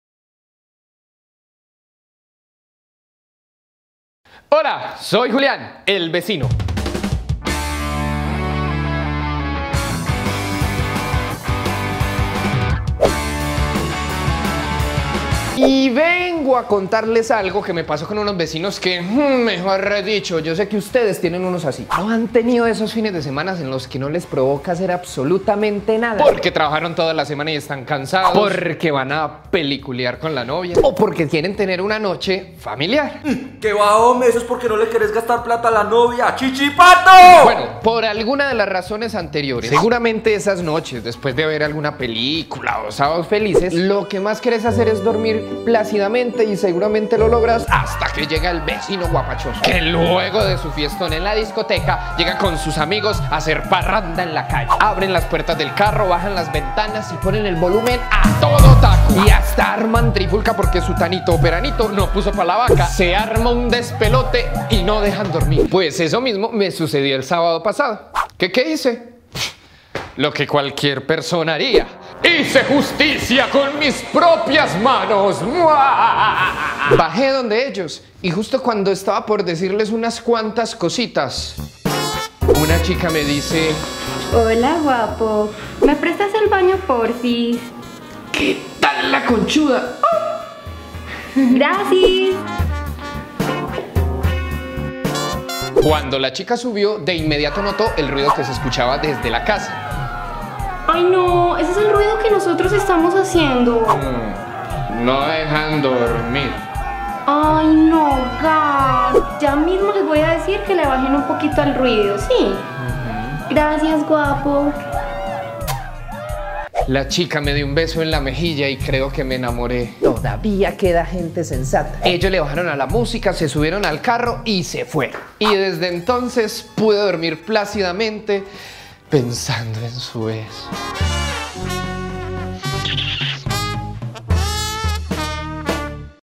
Vecinos guapachosos / Ser un buen vecino es hasta sencillo
Se trata de una iniciativa del componente Medellín Convive la Noche, que busca sensibilizar sobre problemáticas que se generan en este espacio del día.

La iniciativa incluye 6 videos tipo 'Youtubers', denominados “El Vecino”, que tienen como objetivo generar corresponsabilidad entre las personas.